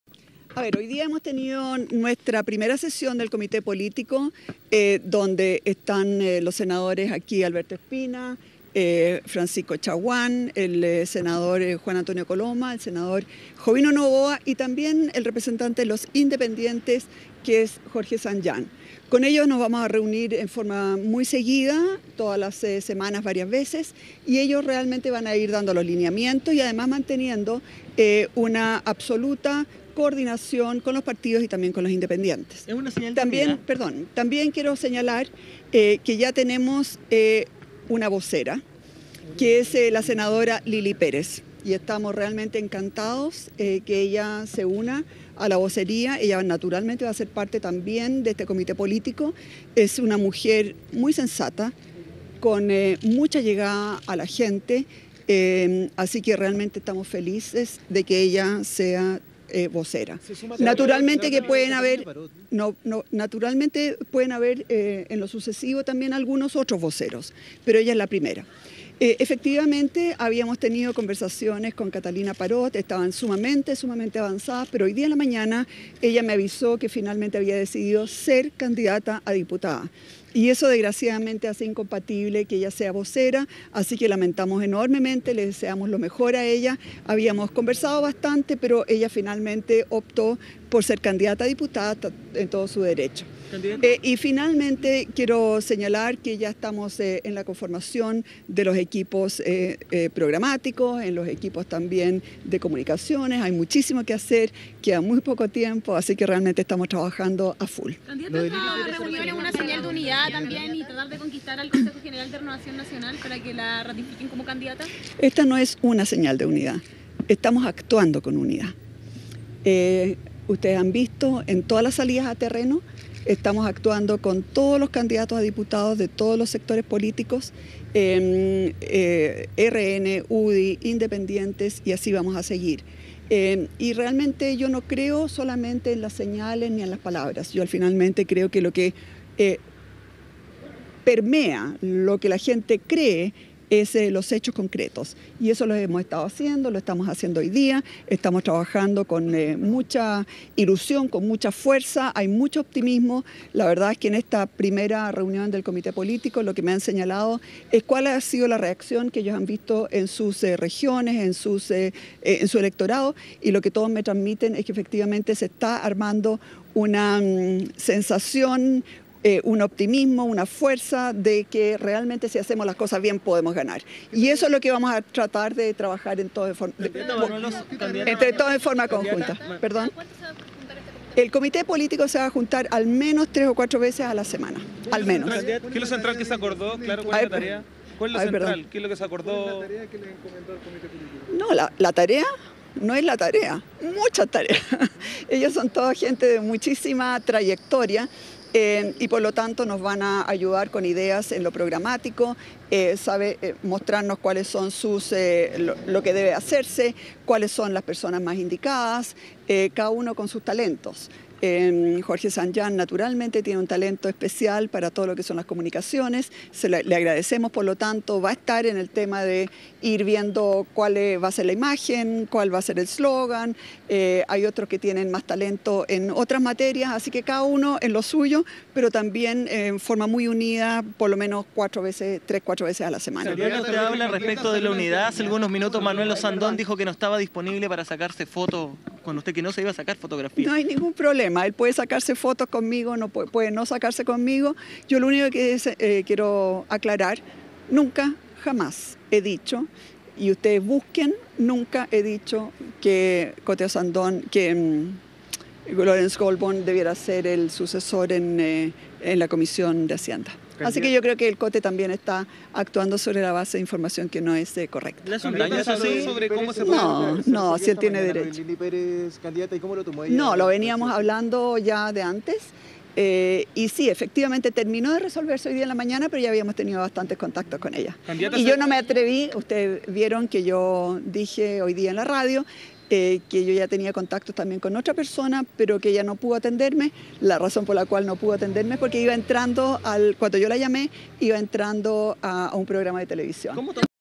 Evelyn Matthei confirma a Lily Pérez como vocera de su campaña presidencial
Inicialmente se esperaba que la ex vocera de Andrés Allamand, Catalina Parot, asumiera este cargo.